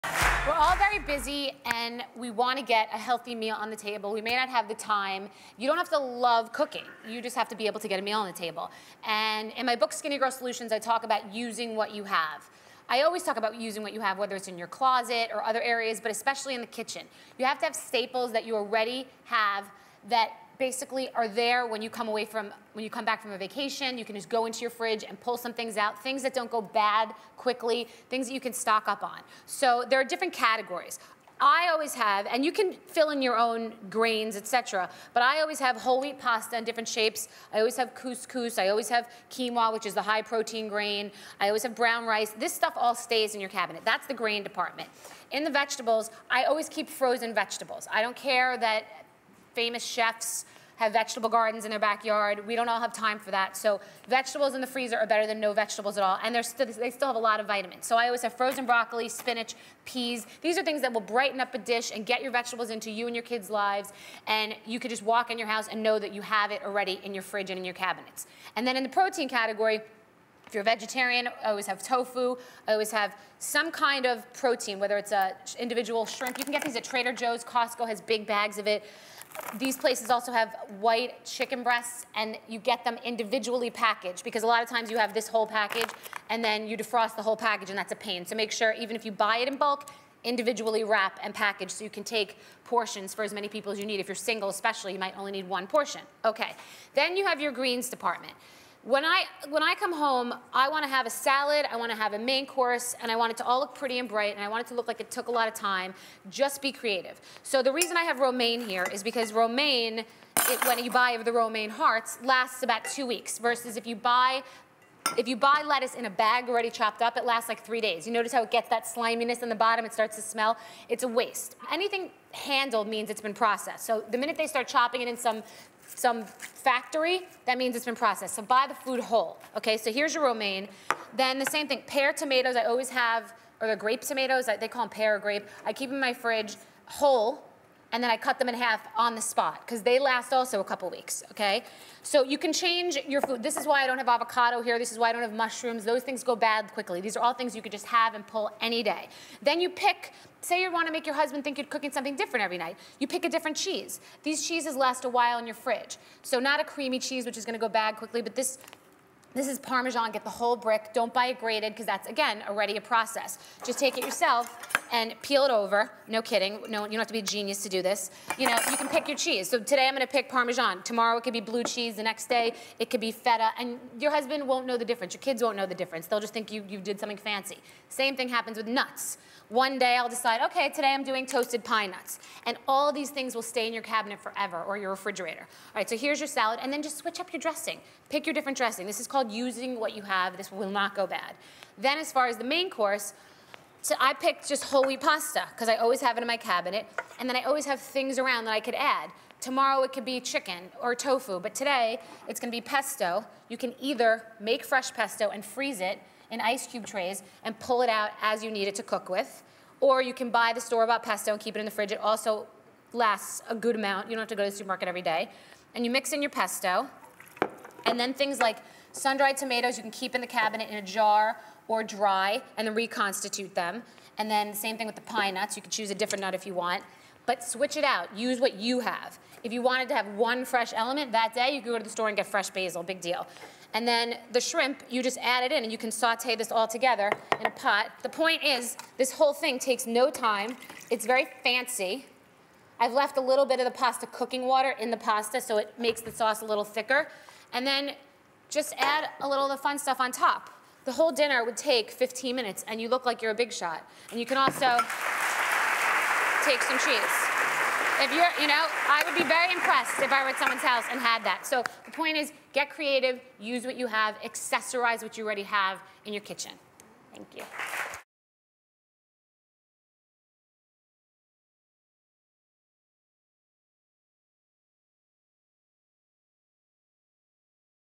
Bethenny's 'Use What You Have' Recipes: Shrimp Pesto Pasta
Bethenny shows you how to whip up an amazingly simple and tasty shrimp pesto pasta.